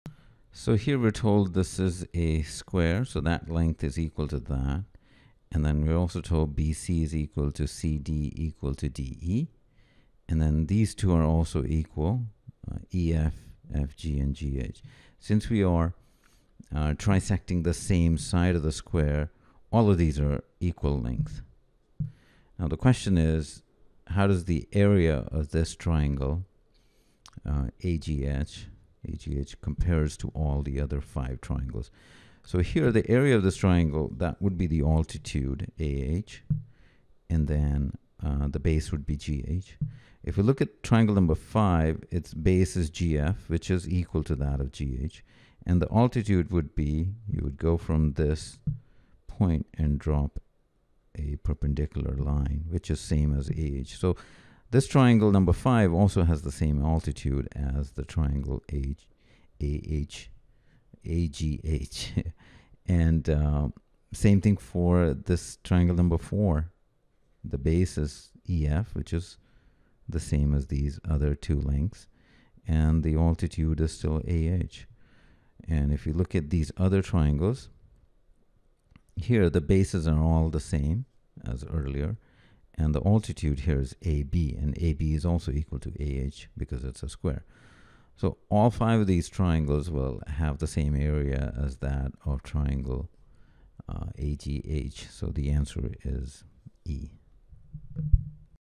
Question 55 2025 February ACT Test Z14 Mathematics
2025 February ACT Form code Z14: Video explanations to all of the 60 math questions in the official 2025 February ACT test.